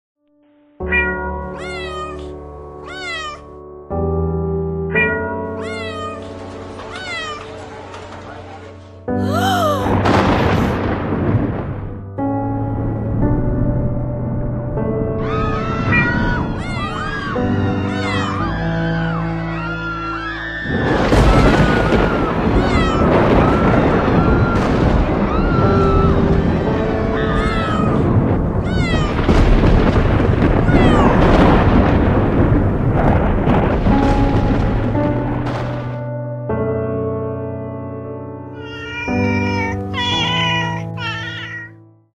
Father and son cats encountered a massive storm at sea
Under a tumultuous sky, a father cat and his young son found themselves at the mercy of a raging tempest while navigating the open sea.